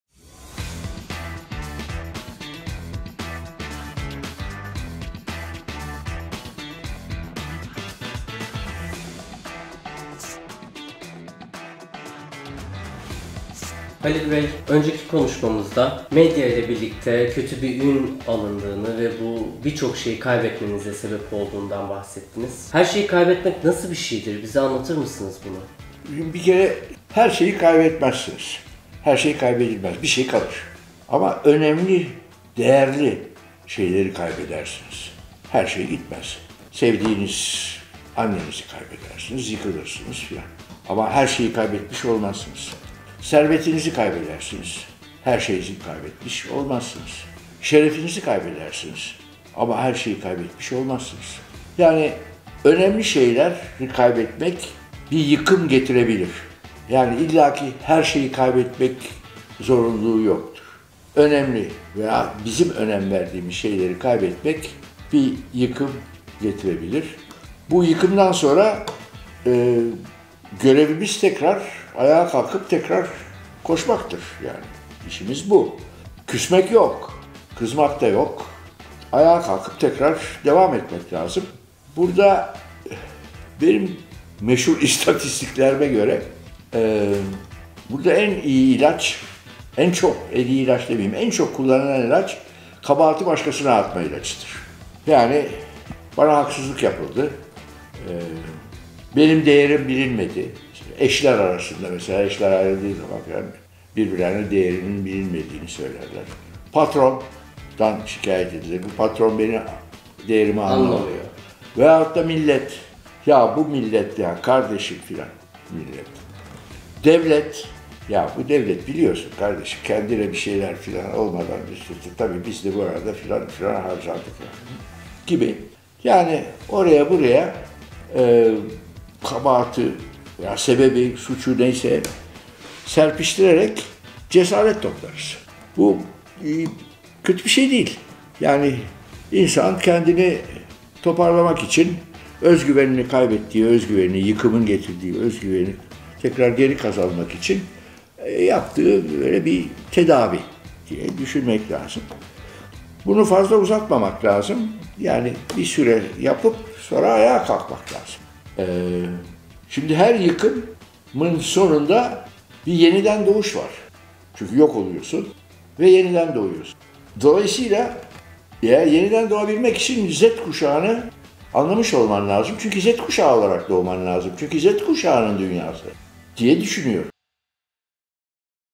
Her şeyi kaybetmek nasıl bir duygu? // Z Kuşağı Soruyor 18 Bölüm
Z kuşağı kısaca sordu ben de kısaca cevap verdim: Her şeyi kaybetmek nasıl bir duygu? Soru zor, fakat cevap bu kadar zor değil...